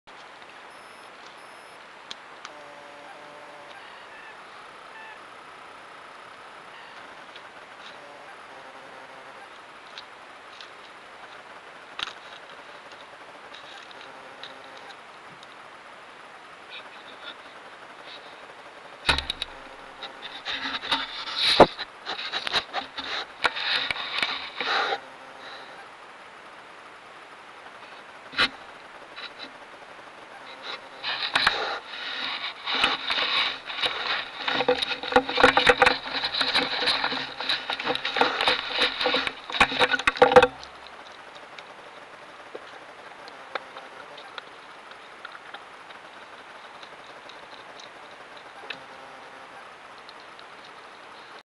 Up close with a Fallow Deer
Fallow deer checking out the game camera.